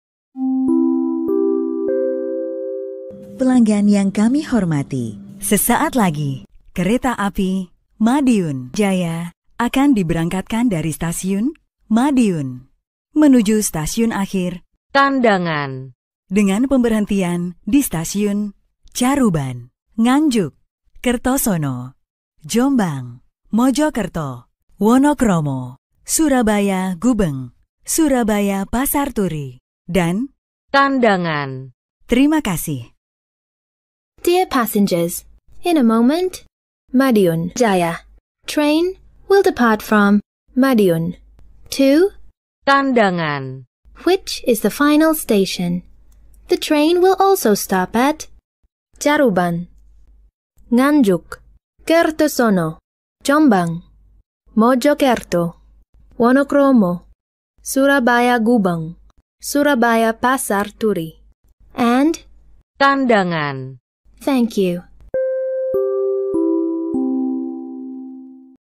Announcer Voice Kereta api Madiun Jaya Kereta api Baru GAPEKA 2021
Kereta api Madiun Jaya merupakan kereta api Fakultatif layanan kelas bisnis dan eksekutif yang memiliki relasi Madiun - Surabaya Pasarturi - Kandangan maupun sebaliknya.

Untuk saat ini KA Madiun Jaya masih berstatus Fakultatif bersama dengan kereta api Lawang Sewu Ekspres, Blambangan Ekspres, Arjuno Ekspres, Jolosutro, Nusa Tembini, Cikuray serta Kamojang.

Sebenarnya tahun lalu telah ada Kereta api Madiun Jaya Ekspres relasi Madiun - Yogyakarta PP namun dihapus dari Perjalanan di GAPEKA 2015 kalo gak salah, akibat okupansi yang sangat minim karena masyarakat lebih banyak berminat di KA Prameks yg saat ini sudah digantikan oleh KRL Commuter Line Jogja - Solo dan rute KA Prameks dipindah ke Yogyakarta - Kutoarjo PP.